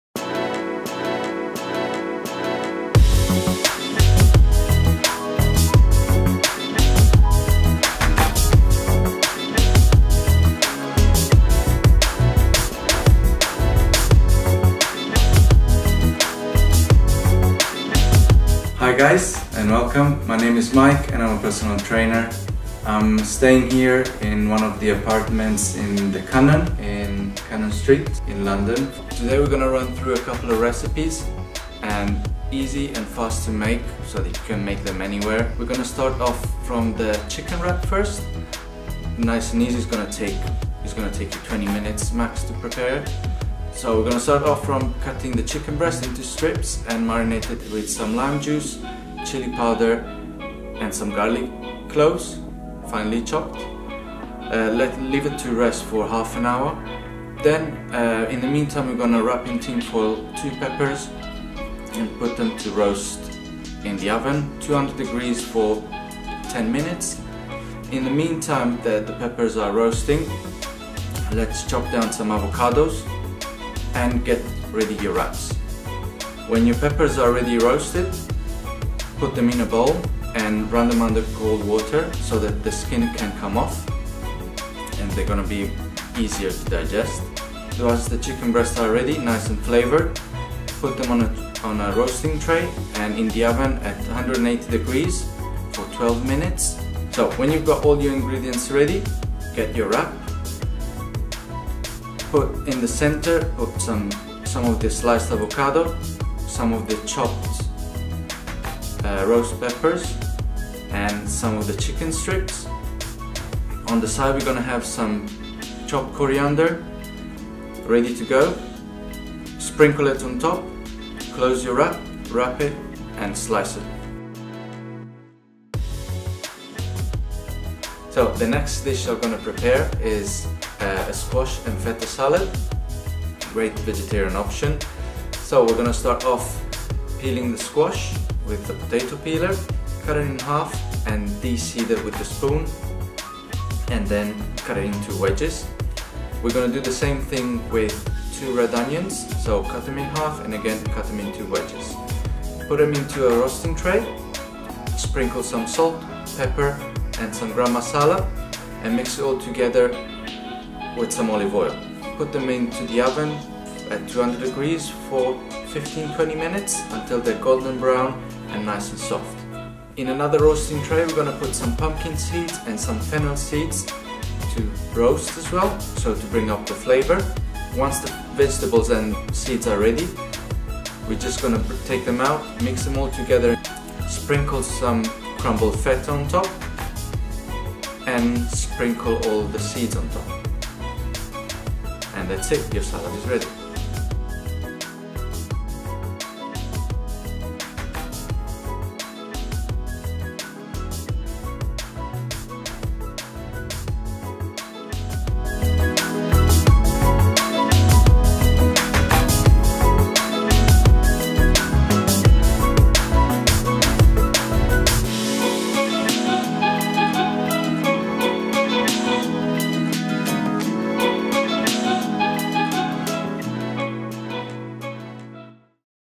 Healthy meals in your Serviced Apartment - Mike cooks at The Cannon
Mike cooks at The Cannon

Hungry? Personal trainer Mike joined us at The Cannon for Wellbeing Wednesday and showed us how to prepare some quick and easy healthy meals! Watch for meal inspiration.